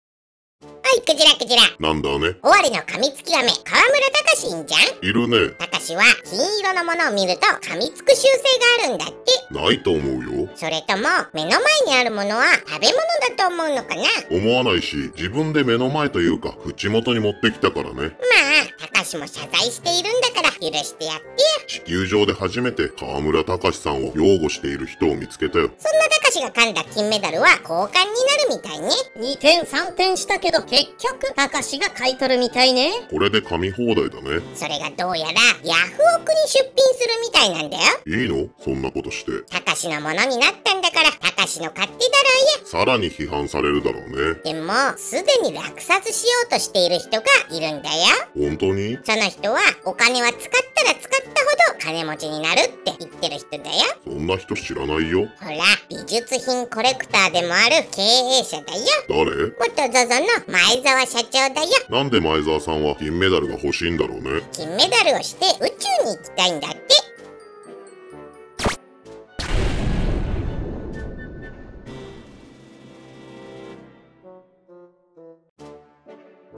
河村たかしが金メダルを買い取った後の予定に一同驚愕‼名古屋市長は辞めさせるべき？【おしえて！くじら先生】
世良さんTwitter始めたってよ